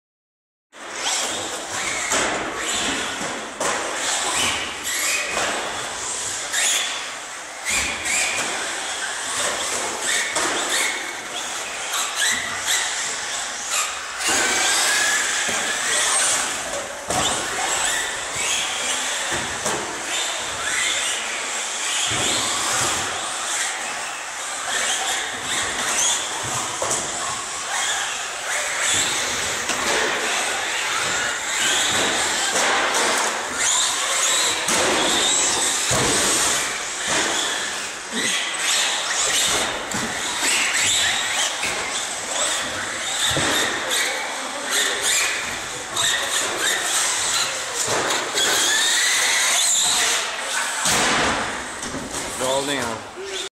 Rynos Racetrack
Film från banan som är hos MiniCars i Enköping.
Vi var där med ett gäng Slash 4x4.

galet kul!